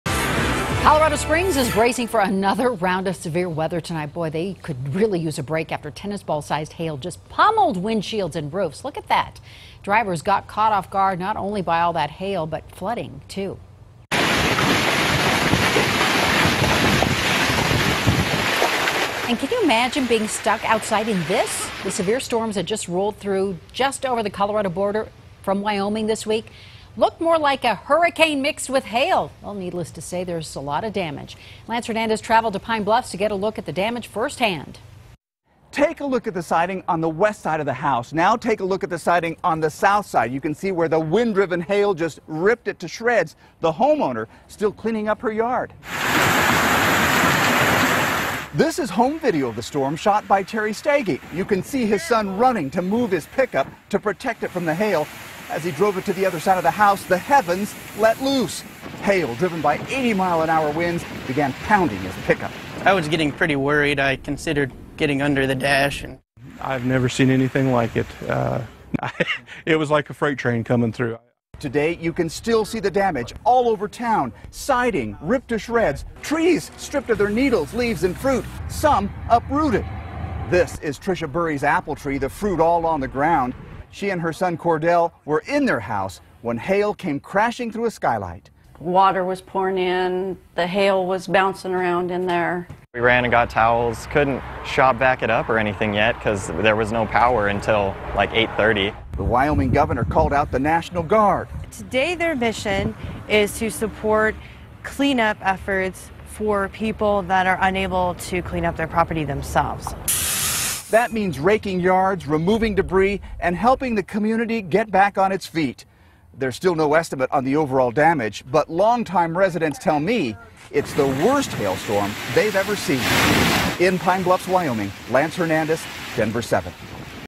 Hailstorm devastates Wyoming town
Denver7 reporter Lance Hernandez traveled to Pine Bluffs to get a look at the damage first-hand.